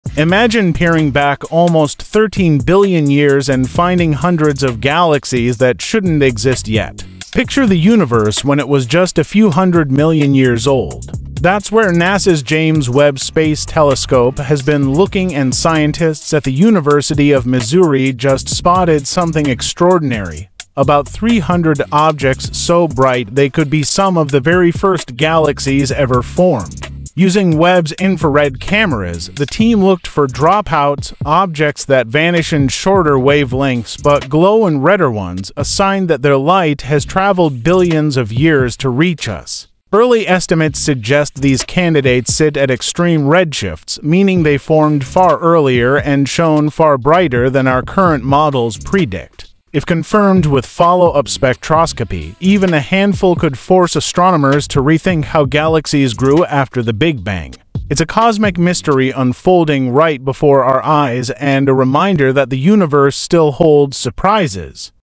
JWST’s 300 Early Galaxies Could Rewrite Cosmic History
Astronomers using JWST spotted ~300 super‑bright objects that may be among the universe’s first galaxies. If confirmed, they could challenge our models of how galaxies formed.